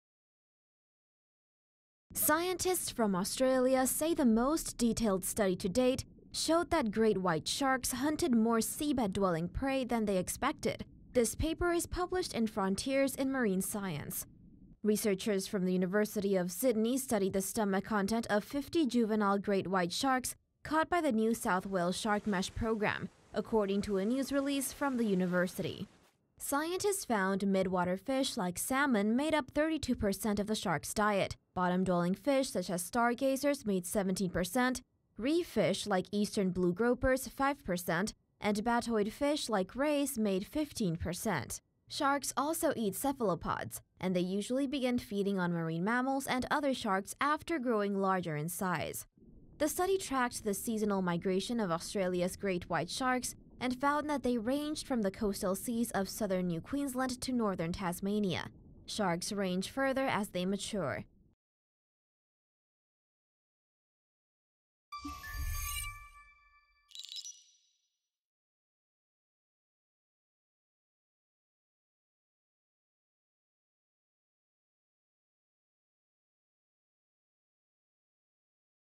Scientists document juvenile great white sharks' diet
Scientists from Australia say the most detailed study to date showed that great white sharks hunted more seabed-dwelling prey than they expected. This paper is published in Frontiers in Marine Science.

Researchers from the University of Sydney studied the stomach content of 50 juvenile great white sharks caught by the New South Wales Shark Mesh Program, according to a news release from the university.

Scientists found mid-water fish like salmon made up 32 percent of the sharks' diet, bottom-dwelling fish such as stargazers made 17 percent, reef fish like Eastern Blue gropers 5 percent, and batoid fish like rays made 15 percent.

Sharks also eat cephalopods, and they usually begin feeding on marine mammals and other sharks after growing larger in size.

The study tracked the seasonal migration of Australia's great white sharks and found that they ranged from the coastal seas of southern New Queensland to northern Tasmania. Sharks range further as they mature.

RUNDOWN SHOWS:
1. Juvenile great white sharks eat more bottom-dwelling fish than expected
2. Researchers studied the stomach content of sharks caught in Australia's shark mesh
3. Breakdown of shark diet by abundance
4. Shark migration path on seas east of Australia